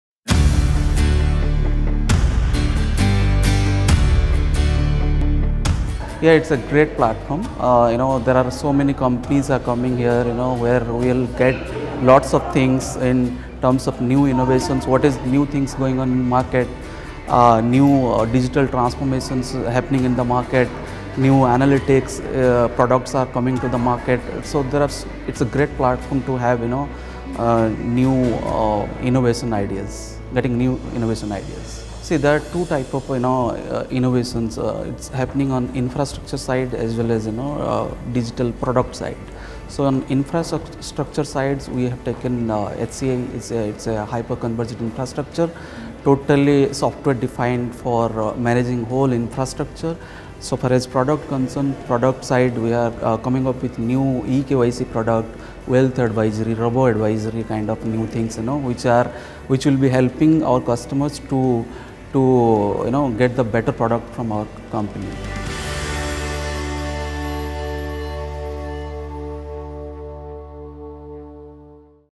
Betting big on digital product: Dipak Rout
Dipak Rout, Head IT, Arihant Capital Markets talks about robo-advisory for better customer experiences.